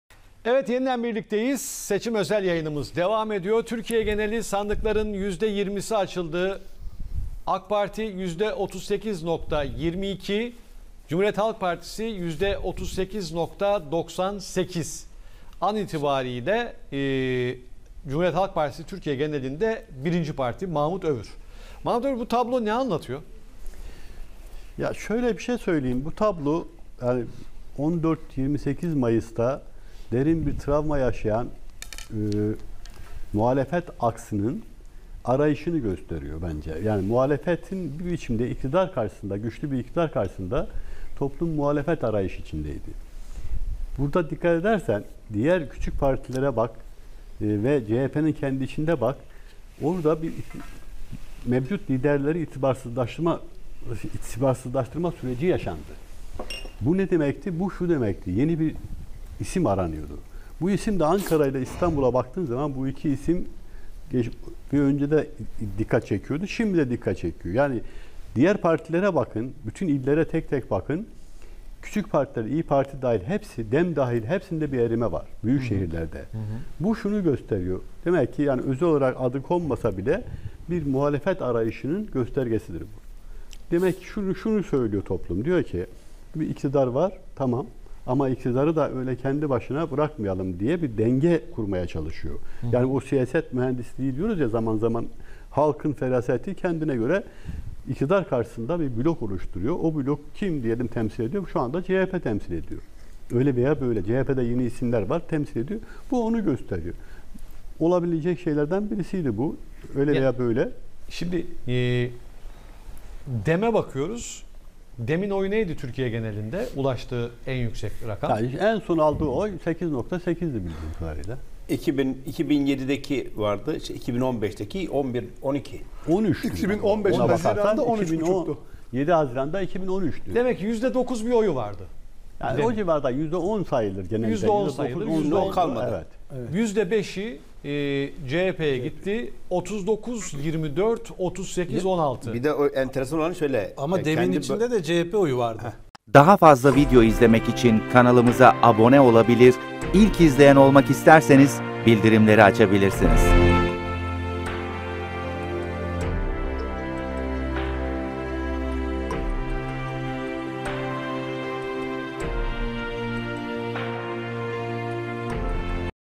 Türkiye'de Tablo Ne Gösteriyor? Seçmen Hangi Mesajı Verdi?
Yerel seçimlerin Türkiye'deki yansımaları ve seçmenin partilere hangi mesajı verdiği canlı yayında değerlendirildi.